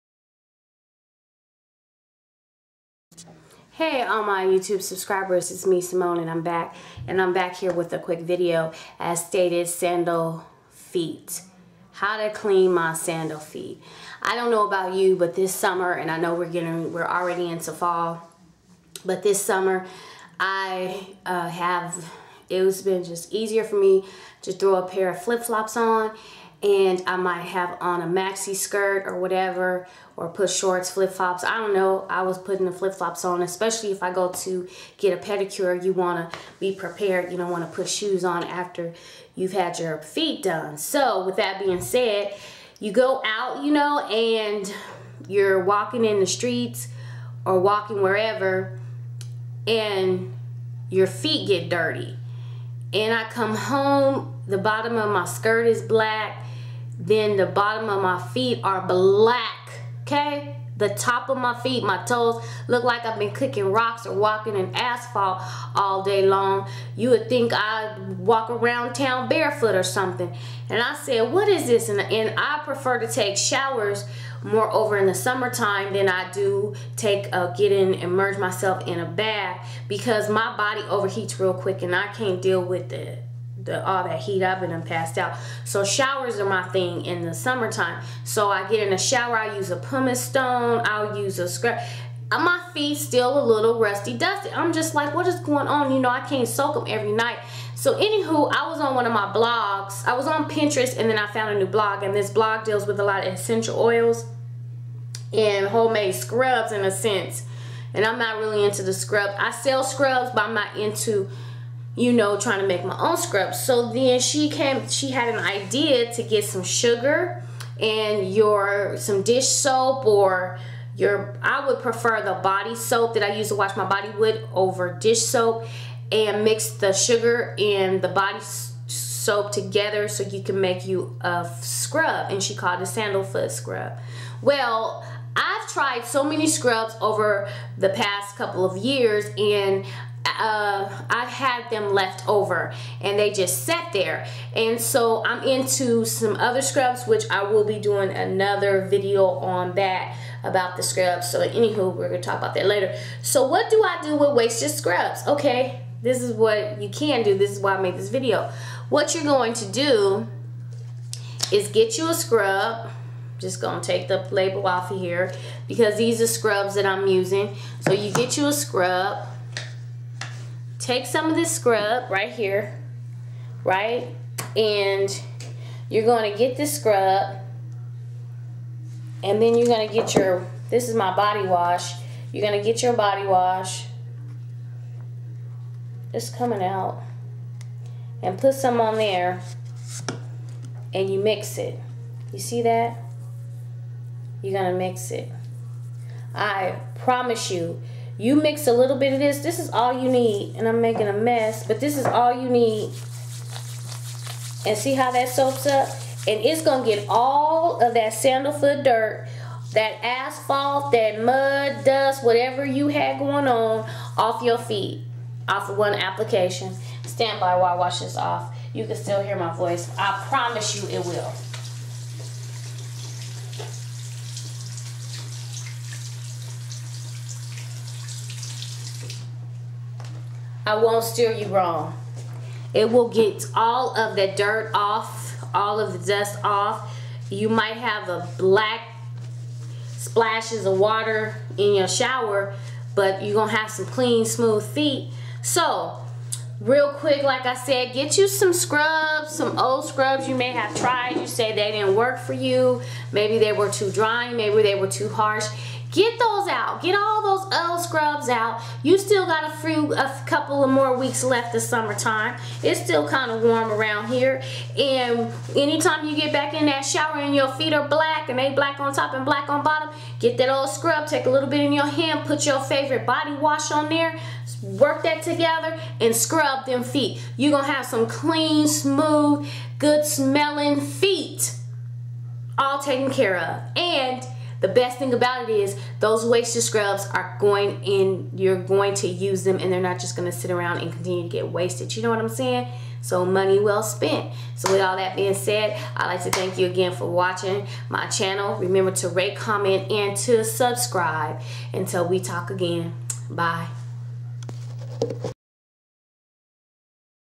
DIY-How To Clean Sandlefeet
Hello! I know. I am way late it is winter time.  However; we are still in the 70's in California but there is not rain.  This can be for the warm days to come.

Items Used:  A scrub that I no longer used that just sat inside the cabinet
Your favorite body wash that you use.  Mix both together and start scrubbing.